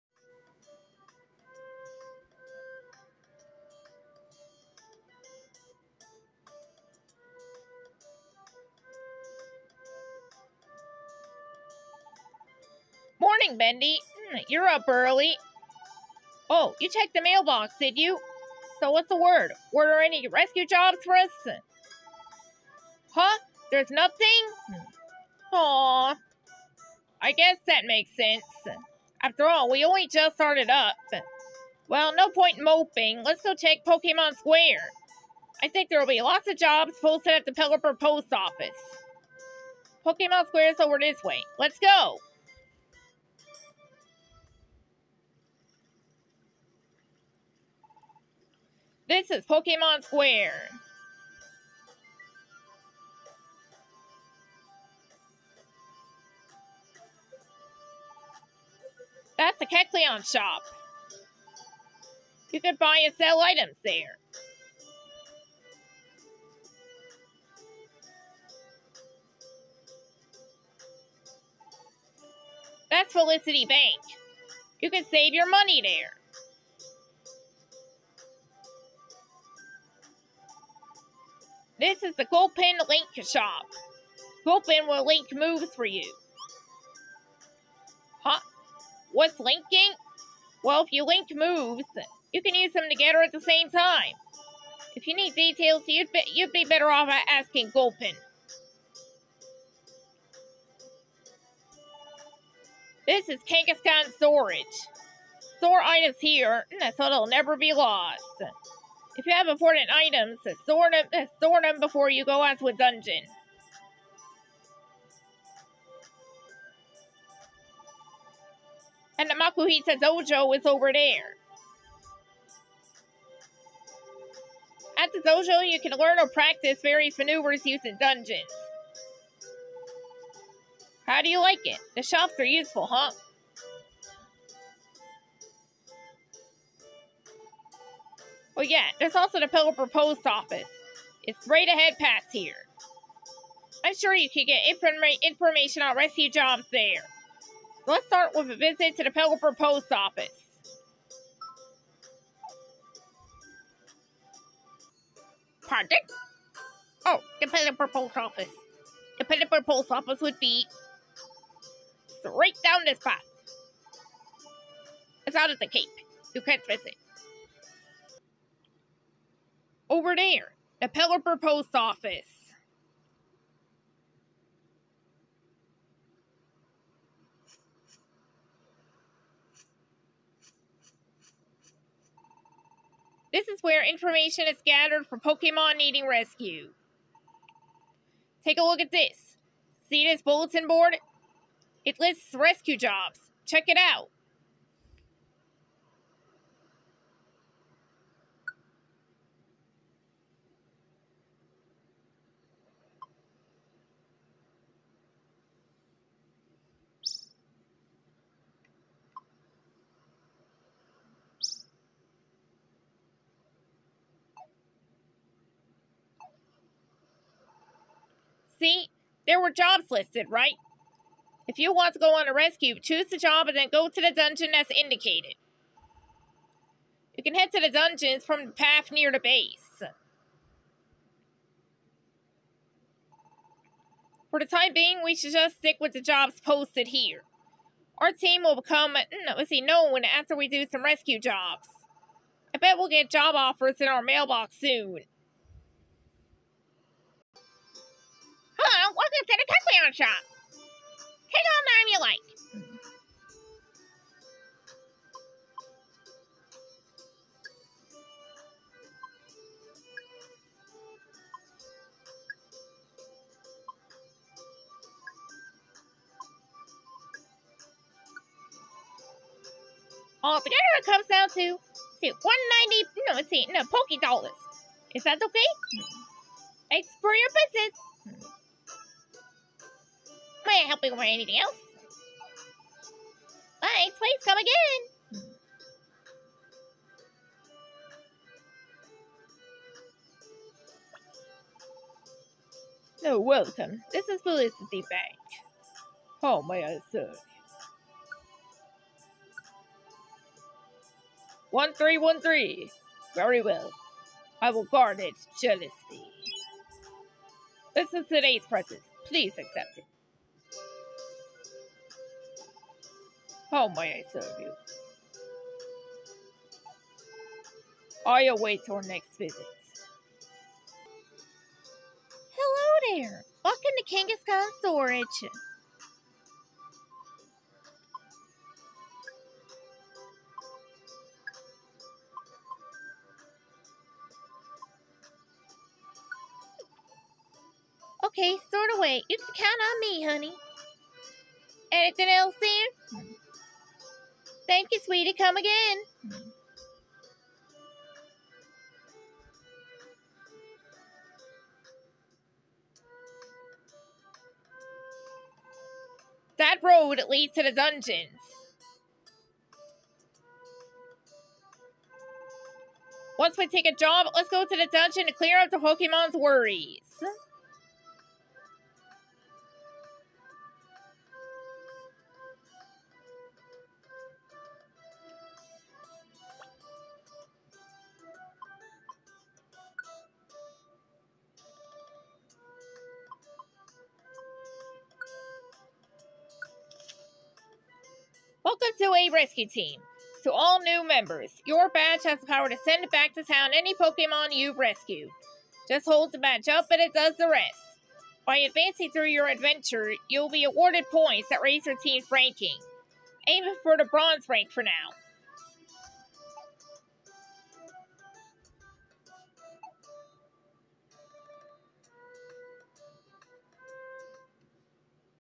Bendy and sora's pokemon adventure episode 3: come on down to pokemon square!
Sora shows bendy the shops of pokemon square and the way to the pelipper post office.

Notes:
1: This is the first video where pokemon square is shown.

Pokedex entries:

Persian kantonian:

Type: normal

Pokedex entry number: 053

Egg group: field

Lombre:

Type Water/grass

Pokedex entry number: 271

Egg group: water 1/grass

Kecleon:

Type: normal

Pokedex entry number: 352

Egg group: field

Bellsprout:

Type: grass/poison

Pokedex entry number: 069

Egg group: Grass

Kangaskhan:

Type: Normal

Pokedex entry number: 115

Egg group: monster

Gulpin:

Type: poison

Pokedex entry number: 316

Egg group: Amorphous

Makuhita:

Type: Fighting

Pokedex entry number: 296

Egg group: human-like

Snubbull:

Type: Fairy (Formerly normal)

Pokedex entry number: 209

Egg group: Field/fairy